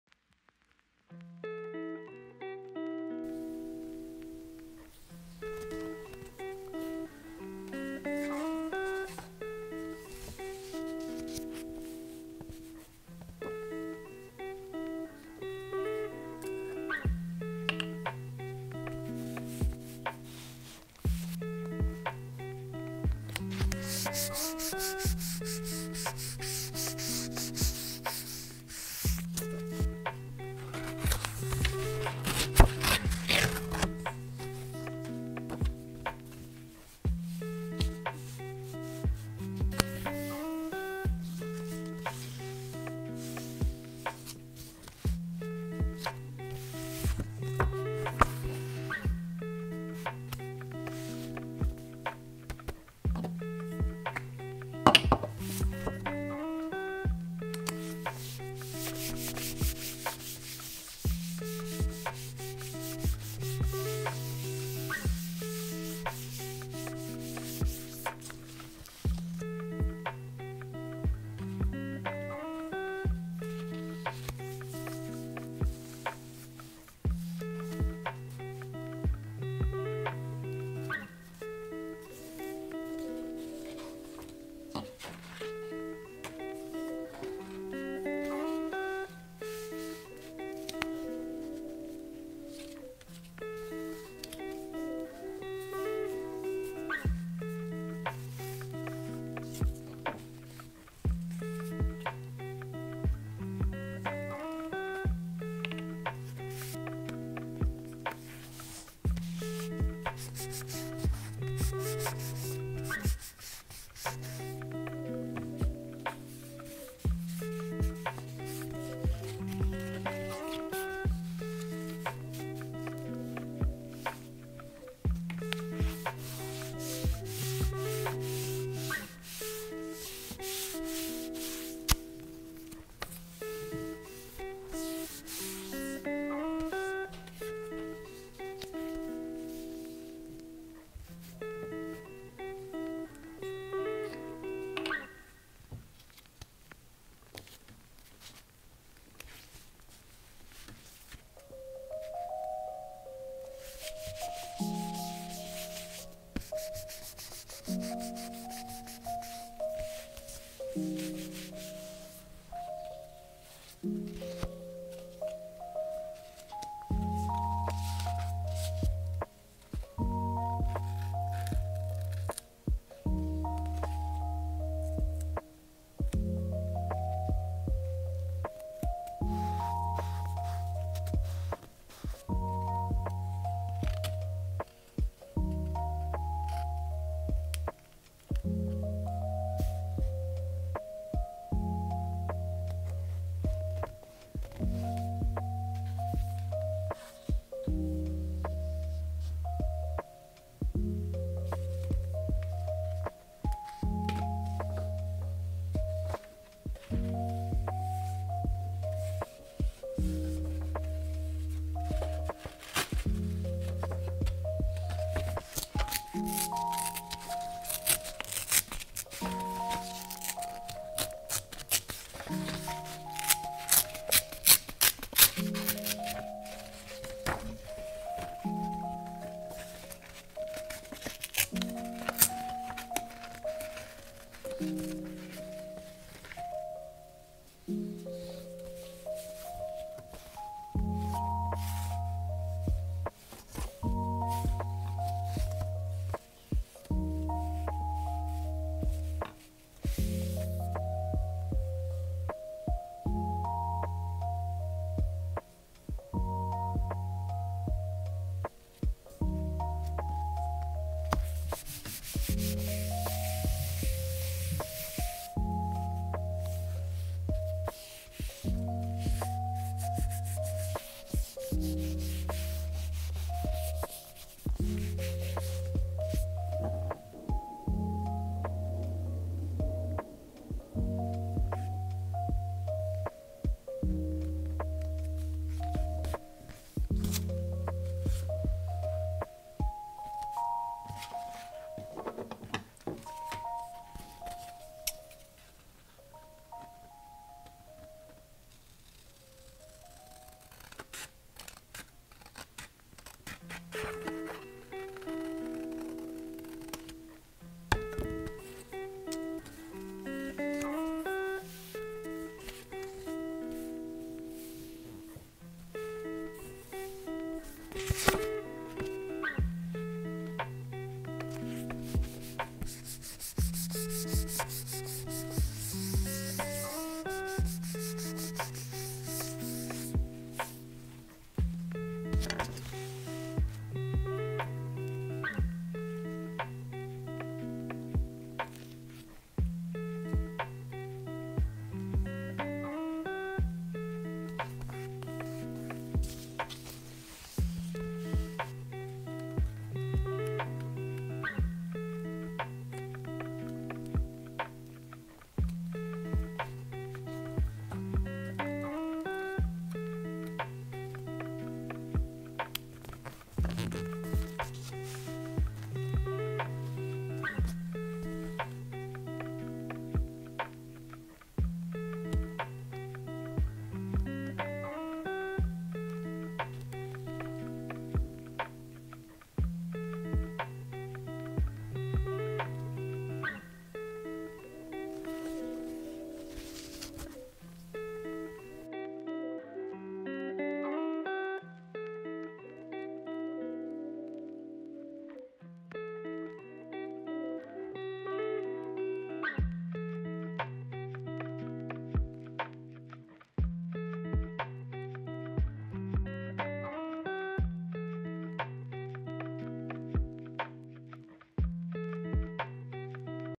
Scrapbook With Me #8 | CC Getting Canceled (asmr, no talking)
Hello!
I hope you have a fantastic day and that this video is interesting to you!
-Peach

You can use this video as background noise while studying or getting some ideas for your own notebook.
Regardless of your reasoning, I’m really grateful you’re here.

Supplies:
-Glue Stick
-Scissors
-Pack(s) of Patterned Paper (from Hobby Lobby)
-Notebook I made from random paper I had lying around

Socials:
Instagram and TikTok: @peachasmrofficial